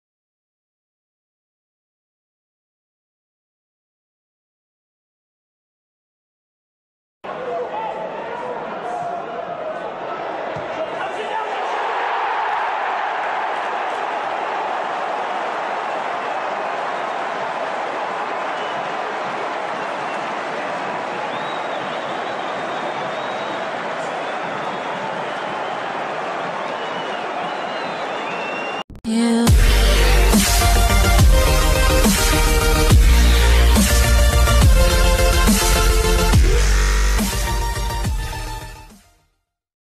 »● Kamil Grosicki goal vs Brentford FC ! 😱«●
Piekna bramka Kamila Grosickiego vs Brentford!
To juz jego 6 bramka w tym sezonie !

Zapraszam do ogladania. 😉😉😉